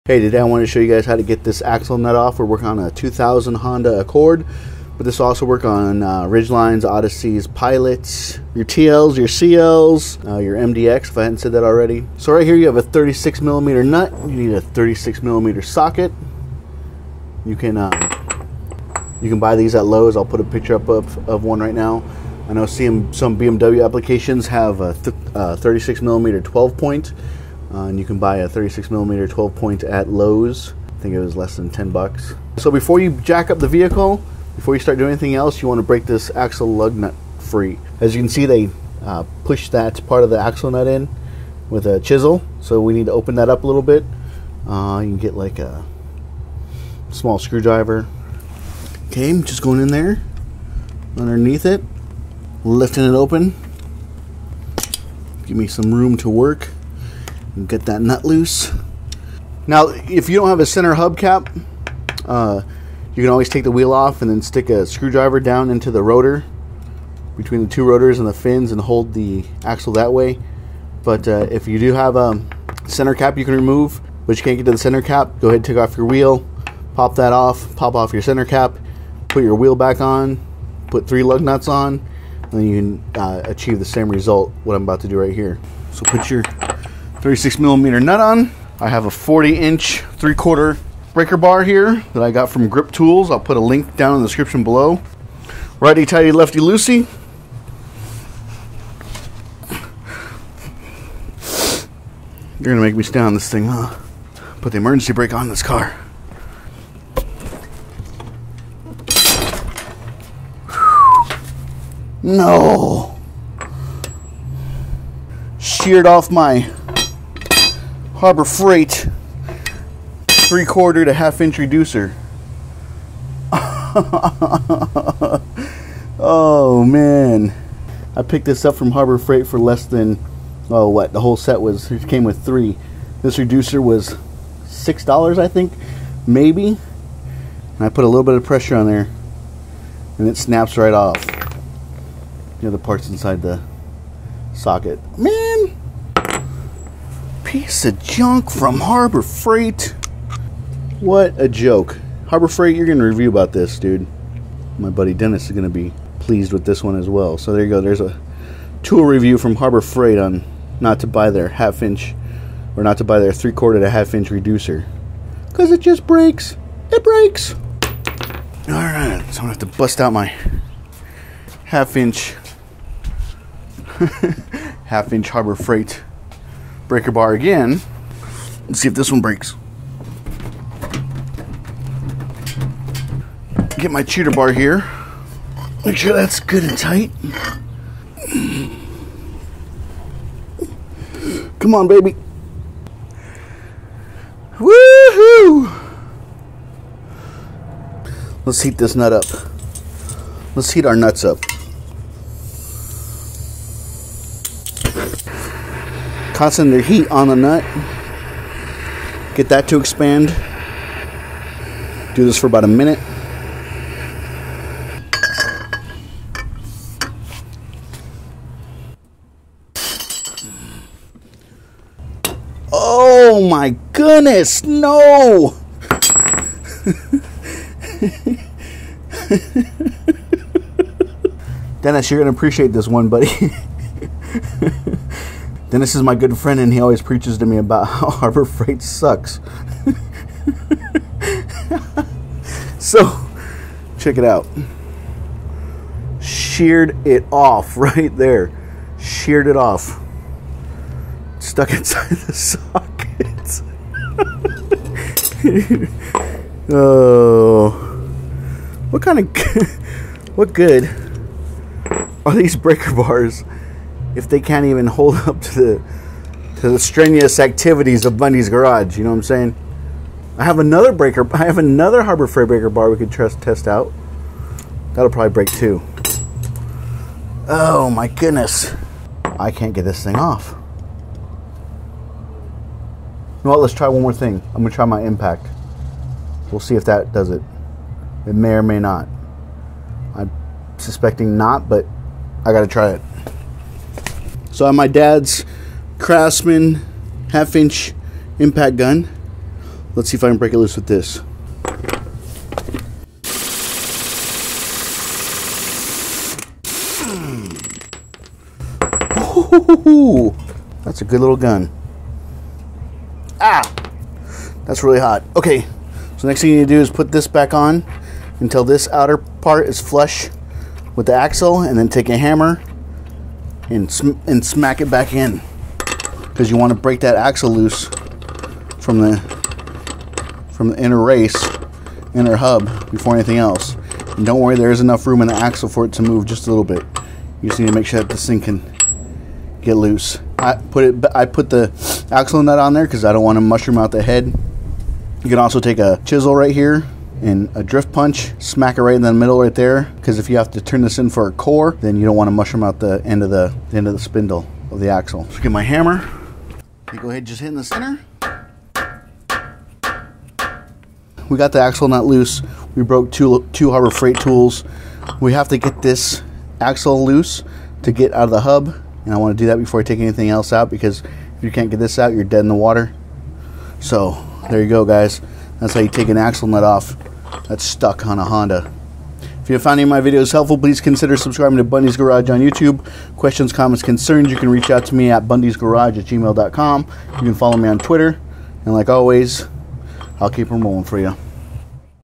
How to Remove Axle Nut on Honda - Rusted Stuck Axle Nut will not come off - Bundys Garage
Bundy's adventure in getting off a stuck axle nut. How to Remove Axle Nut not easy. Piece of junk Harbor Freight tools, fail on me and I have to go to my Craftsman impact gun that keeps on giving.

Bundys Garage